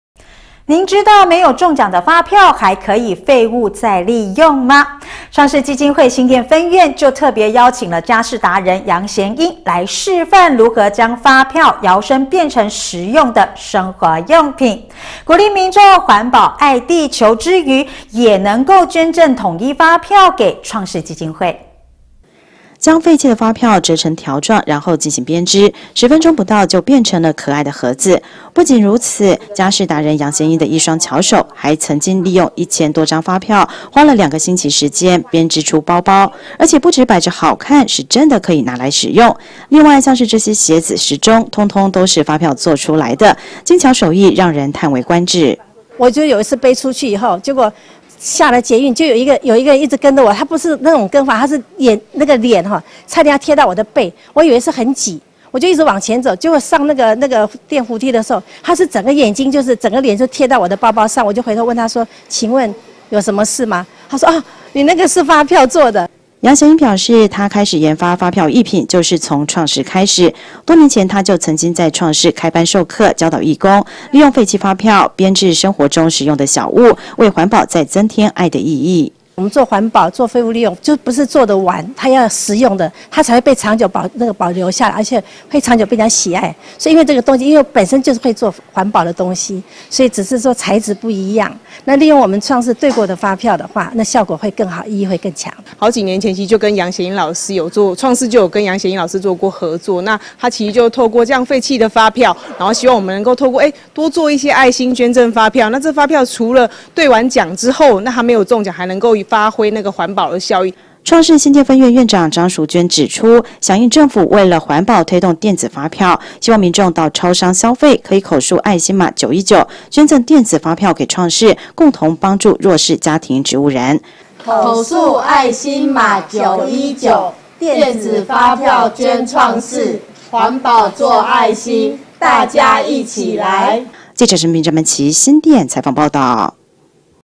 1070130【大新店地方新聞】廢棄發票變身包包鞋子 環保又有愛
你知道沒中獎的發票還可以廢物再利用嗎?創世基金會新店分院特別邀請家事達人楊賢英示範如何將發票，搖身變成實用的生活用品。鼓勵民眾環保愛地球，同時提醒民眾捐贈電子發票給創世基金會。